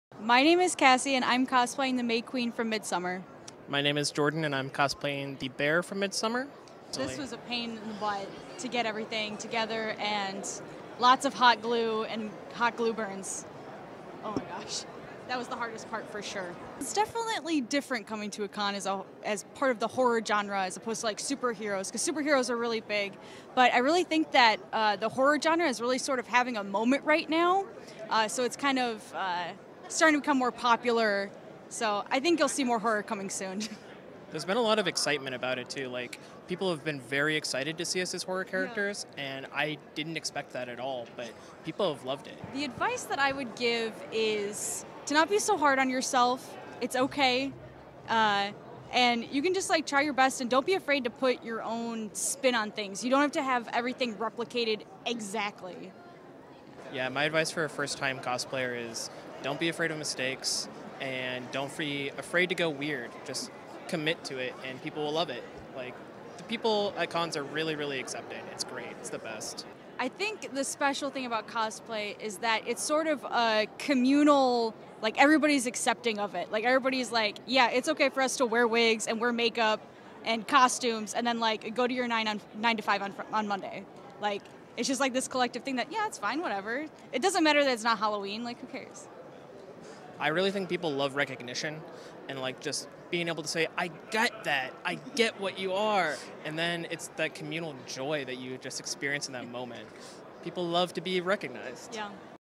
Once Upon a Cosplay: Midsommar Horror
It’s not every day that you see horror cosplays on the con floor, and these Midsommar cosplays are no exception! Both cosplayers decided to cosplay as the May Queen and the Bear. They describe how their feelings wearing horror cosplays at a comic con convention started out uncertain, but turned into a fun, memorable experience that they will continue to remember for years to come.